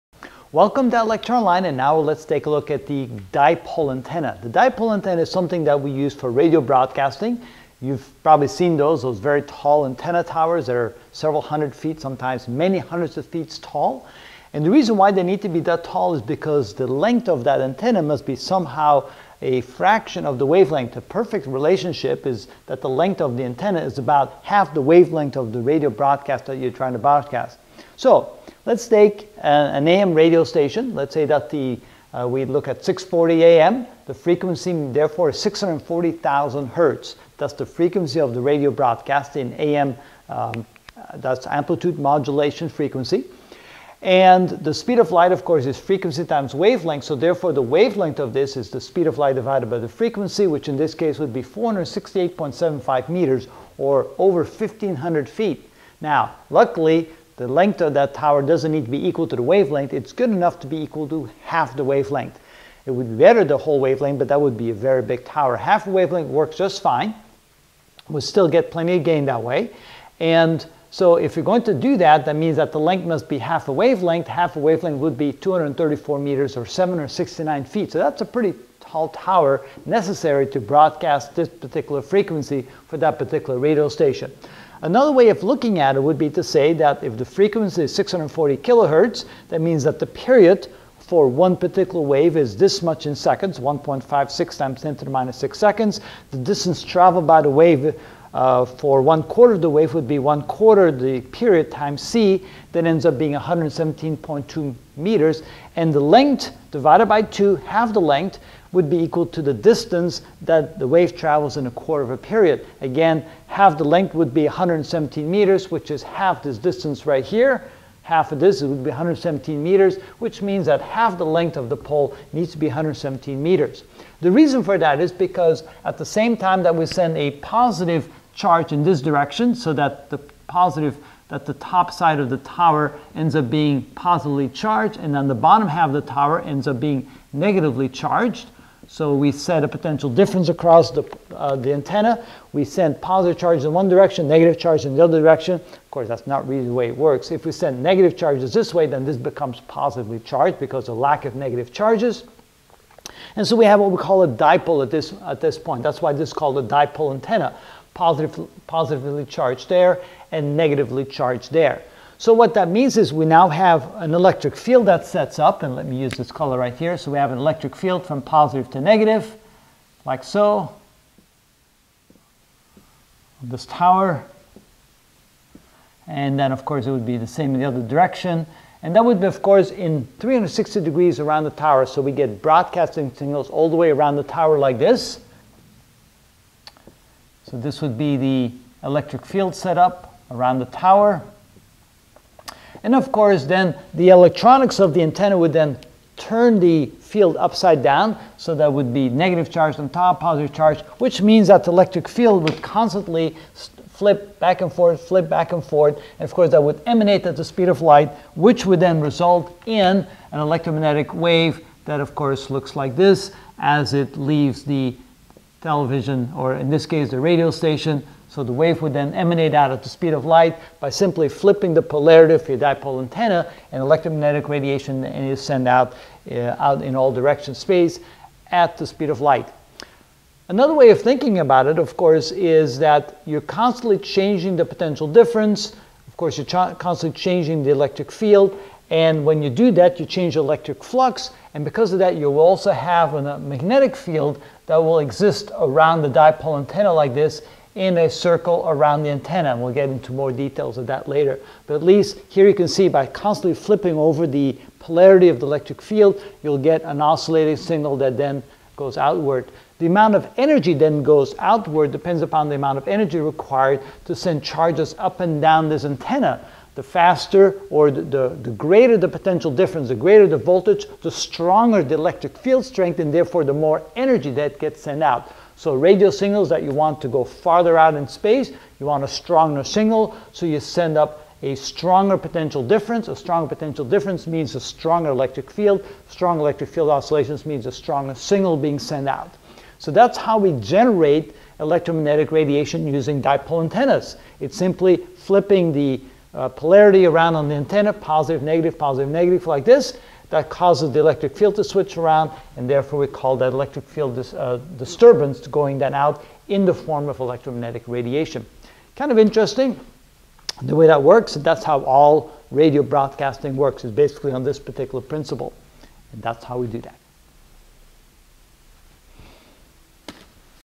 Physics 50 E&M Radiation (6 of 33) Dipole Antenna and E&M Radiation
In this video I will explain electromagnetic radiation and the dipole antenna (AM radio).